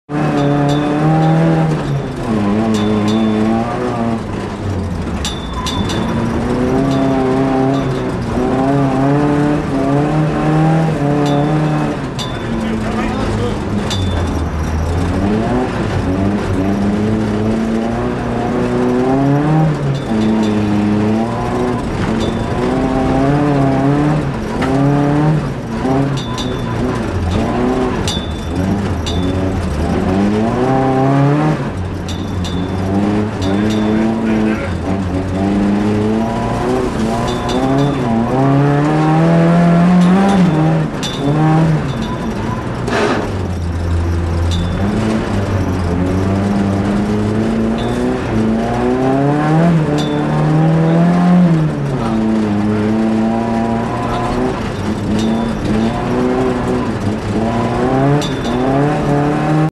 Giro in pista su ghiaccio ad Auronzo ICE-KART
un giro nella pista di Auronzo su Fiat Uno gr S  equipaggio LORO-MARANGON 23/01/11